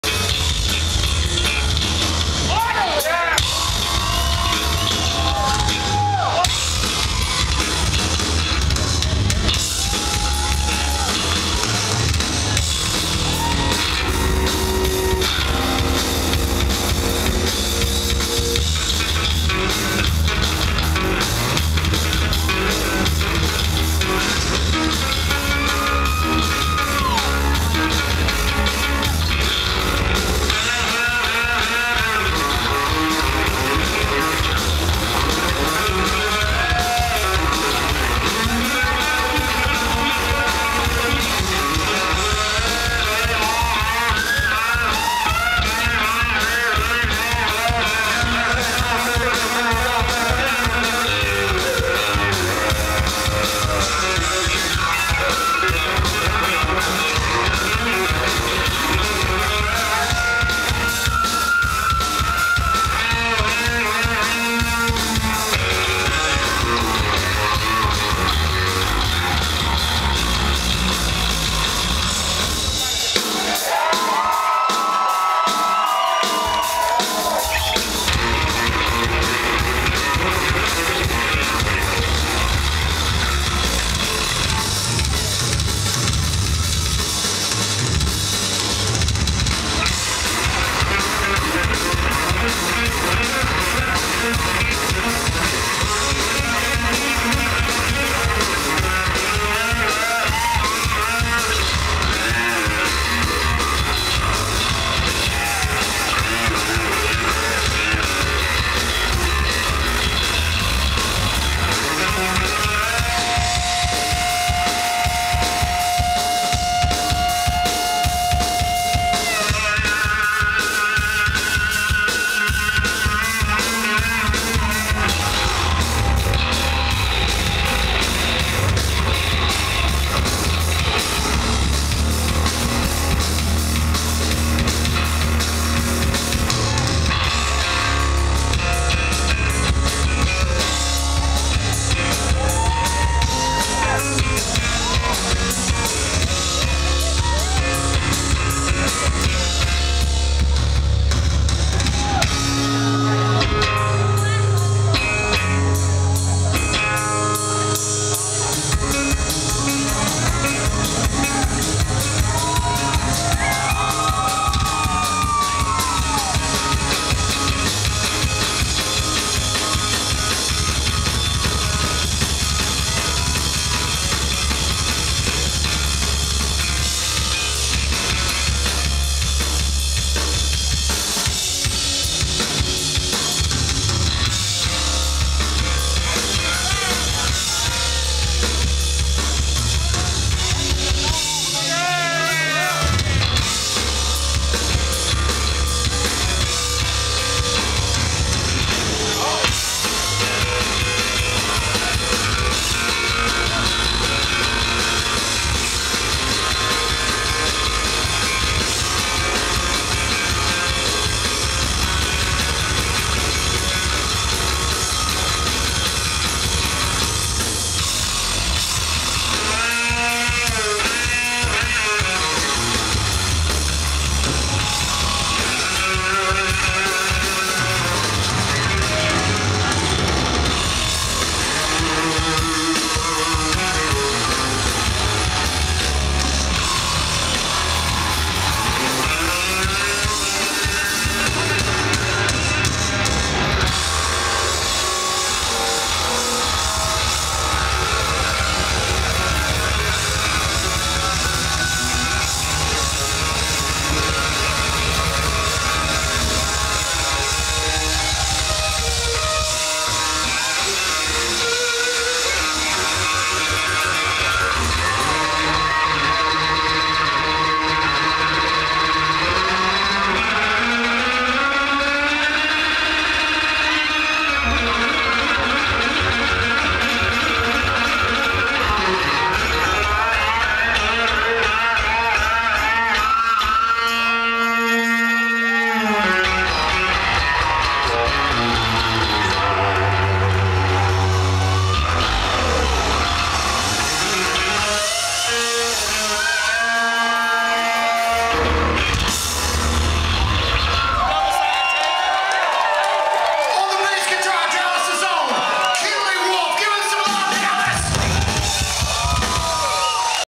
Lord Tracy Barney Bass Solo
Kinley 'Barney' Wolfe  plays with Lord Tracy at the House of Blues -Dallas , Texas ,May 22, 2010.